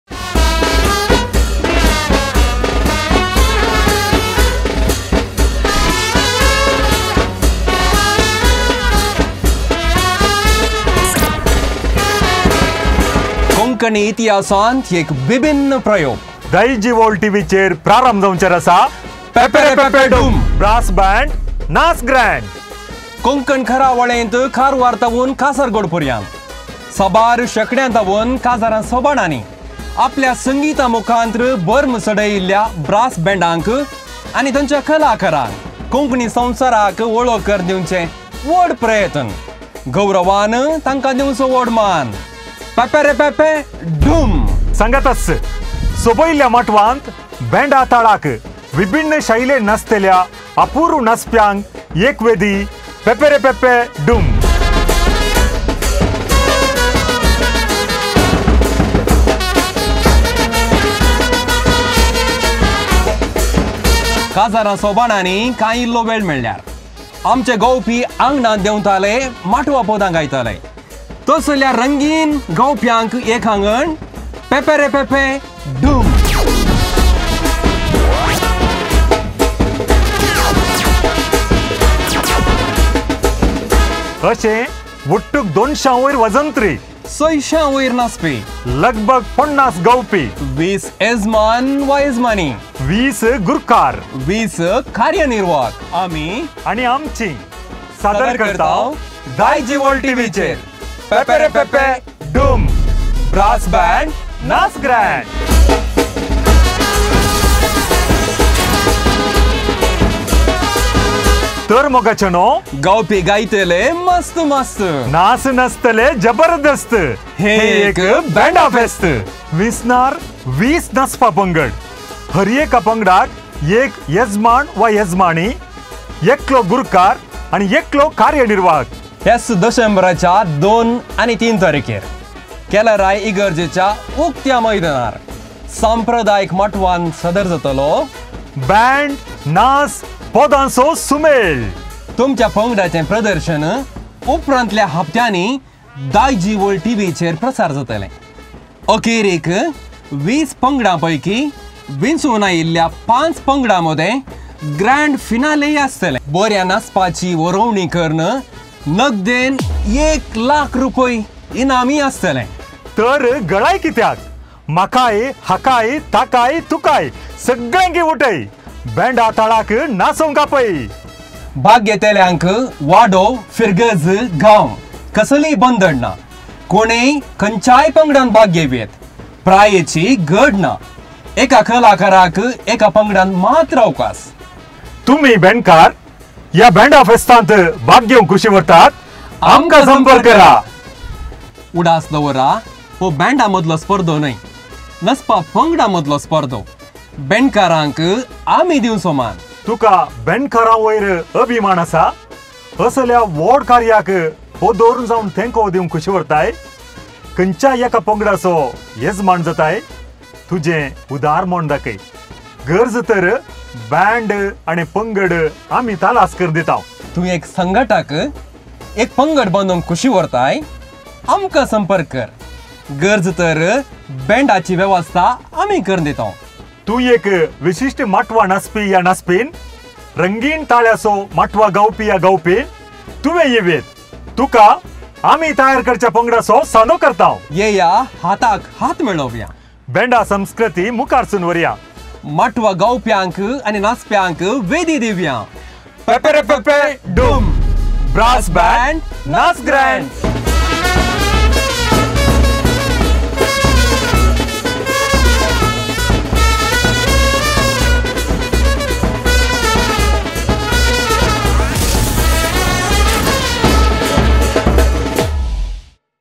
Promo:ಪೆಪೆರೆ ಪೆಪೆ ಢುಂ- Band Fest at Kelarai Church ground on 2nd and 3rd Dec. 2023│Pepere Pepe Dhum
ಹ್ಯಾಚ್ ದಸೆಂಬರಾಚಾ ದೋನ್ ಆನಿಂ ತೀನ್ ತಾರಿಕೆರ್ . ಕೆಲ್ರಾಯ್ ಫಿರ್ಗಜೆಚಾ ಉಗ್ತ್ಯಾ ಮೈದಾನಾರ್, ಸಾಂಪ್ರಾದಾಯಿಕ್ ಮಟ್ವಾಂತ್ ಸಾದರ್ ಜಾತಾಲೊ ,
ಬೇಂಡಾ ನಾಚ್, ಪದಾಂಚೊ ಸುಮೇಳ್.
ತುಮ್ಚ್ಯಾ ಪಂಗ್ಡಾಚೆಂ ಪ್ರದರ್ಶನ್ ಉಪ್ರಾಂತ್ಲ್ಯಾ ಹಫ್ತ್ಯಾನಿಂ  ದಾಯ್ಜಿವರ್ಲ್ಡ್ ಟಿವಿಚೆರ್ ಪ್ರಸಾರ್ ಜಾತೆಲೆಂ.
ಅಖೇರಿಕ್ ವೀಸ್ ಪಂಗ್ಡಾಂಪಯ್ಕಿಂ , ವಿಂಚೊನ್ ಆಯ್ಲ್ಯ ಪಾಂಚ್ ಪಂಗ್ಡಾಂ ಮಧೆಂ ಗ್ರ್ಯಾಂಡ್ ಫಿನಾಲೆ ಅಸ್ತೆಲೆಂ.
ಬರ್ಯಾ ನಾಚ್ಪಾಚಿ ವರವ್ಣಿ ಕರ್ನ್ ನಗ್ದೆನ್ ಏಕ್ ಲಾಕ್ ರುಪಯ್ ಇನಾಮ್ ಯೀ ಅಸ್ತೆಲೆಂ.

ಭಾಗ್ ಘೆತೆಲ್ಯಾಂಕ್ ವಾಡೊ, ಫಿರ್ಗಜ್, ಗಾಂವ್ ಕಸಲಿಯ್ ಬಂಧಡ್ ನಾ. ಕೊಣೆಂಯ್ ಖಂಚ್ಯಾಯ್ ಪಂಗ್ಡಾಂತ್ ಭಾಗ್ ಘೆವ್ಯೆತ್. ಪ್ರಾಯೆಚಿ ಗಡ್ ನಾ. ಎಕಾ ಕಲಾಕರಾಕ್ ಎಕಾ ಪಂಗ್ಡಾಂತ್ ಮಾತ್ರ್ ಅವ್ಕಾಸ್.

ತುಕಾ ಬೆಂಡ್ಕರಾಂ ವಯ್ರ್ ಅಭಿಮಾನ್ ಆಸಾ.?
ಅಸಲ್ಯಾ ವ್ಹಡ್ ಕಾರ್ಯಾಕ್ ಪೊದೊರ್ನ್ ಜಾವ್ನ್  ತೆಂಕೊ ದೀಂವ್ಕ್ ಖುಶಿ ವ್ಹರ್ತಾಯ್..?
ಖಂಚ್ಯಾಯ್ ಎಕಾ ಪಂಗ್ಡಾಚೊ ಯೆಜ್ಮಾನ್ ಜಾತಾಯ್..?
ತುಜೆಂ ಉದಾರ್ ಮನ್ ದಾಕಯ್.
ಗರ್ಜ್ ತರ್ ಬೇಂಡ್ ಆನಿಂ ಪಂಗಡ್ ಆಮಿಂ ತಲಾಸ್ ಕರ್ನ್ ದಿತಾಂವ್.

ತುಂ ಏಕ್ ಸಂಘಾಟಕ್..?
ತುಂ ಏಕ್ ಪಂಗಡ್ ಭಾಂದುಂಕ್ ಖುಶಿ ವ್ಹರ್ತಾಯ್..?
ಆಮ್ಕಾಂ ಸಂಪರ್ಕ್ ಕರ್.
ಗರ್ಜ್ ತರ್ ಬೇಂಡಾಚಿ ವ್ಯವಸ್ಥಾ ಕರ್ನ್ ದಿತಾಂವ್.

ತುಂ ಏಕ್ ವಿಶಿಷ್ಟ್ ಮಾಟ್ವಾ ನಾಚ್ಪಿ?
ರಂಗೀನ್ ತಾಳ್ಯಾಚೊ ಮಾಟ್ವಾ ಗಾವ್ಪಿ?
ತುವೆಂಯ್ ಯೆವ್ಯೆತ್.
ಆಮಿಂ ತಯಾರ್ ಕರ್ಚ್ಯಾ ಪಂಗ್ಡಾಚೊ ಸಾಂದೊ ಕರ್ತಾಂವ್.